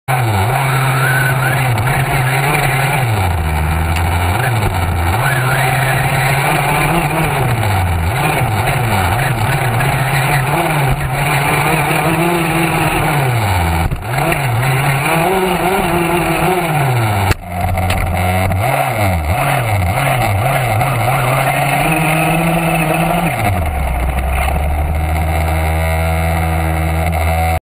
3-D GoPro onboard HPI Baja 5t trial
this is  clip of a video i am working from the phatoberfest at the phatrack.